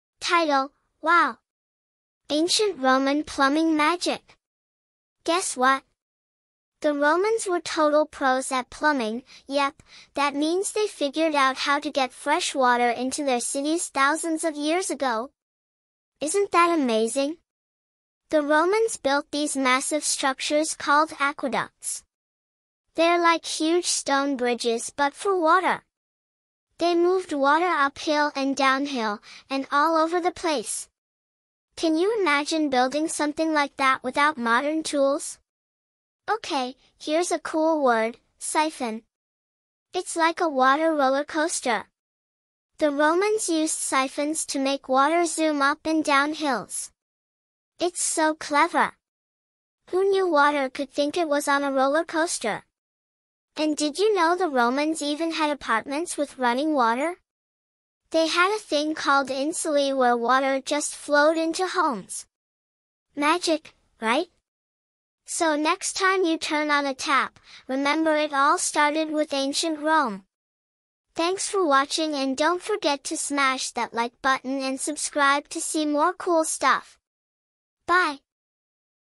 The TRUTH About Ancient Rome's Incredible Innovations - Explained Simply!
Title: "Wow! Ancient Roman Plumbing Magic!"
- Guess what?! The Romans were total pros at plumbing—yep, that means they figured out how to get fresh water into their cities thousands of years ago! Isn't that amazing?
- The Romans built these massive structures called aqueducts. They're like huge stone bridges but for water! They moved water uphill and downhill, and all over the place! Can you imagine building something like that without modern tools?
- Okay, here's a cool word: siphon. It’s like a water rollercoaster! The Romans used siphons to make water zoom up and down hills. It's so clever! Who knew water could think it was on a rollercoaster?
- And did you know the Romans even had apartments with running water?! They had a thing called insulae where water just flowed into homes! Magic, right?
- So next time you turn on a tap, remember it all started with ancient Rome! Byeee!

Ancient Rome's Incredible Innovations | Curious Little Kiddo

Learn amazing facts about ancient rome's incredible innovations explained in a fun, simple way!
Perfect for curious minds of all ages.